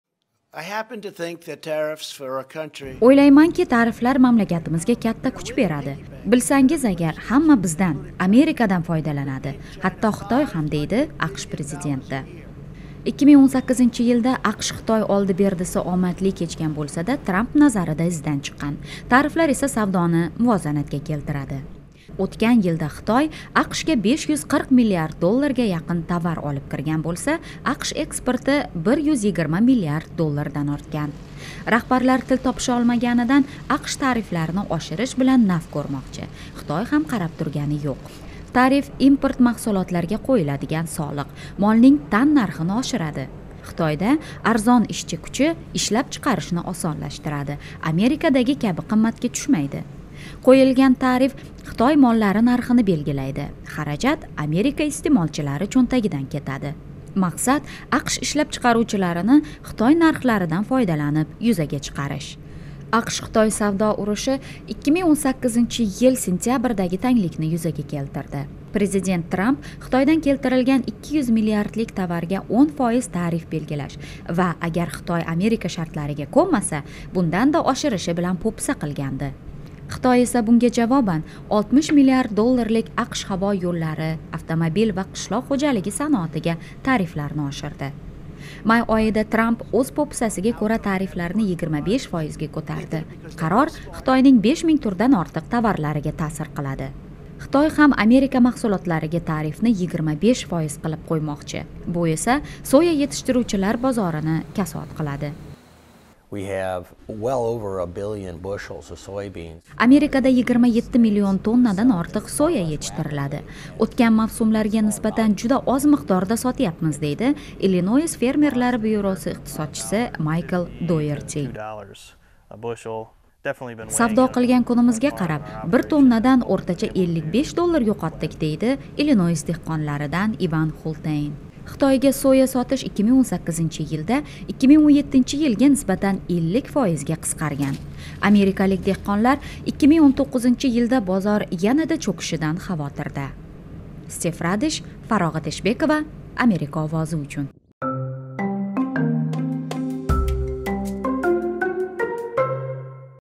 AQSh-Xitoy savdo urushi: Oshirilgan tariflar kuchga kirishi mumkin
AQSh moliya vaziriga ko’ra, Xitoy mahsulotlariga qo’yiladigan tariflar kamida bir oy ichida oshadi. Ikki davlat rahbarlari til topisholmaganidan AQSh tariflarni oshirish bilan naf ko’rmoqchi. Xitoy ham qarab turgani yo’q.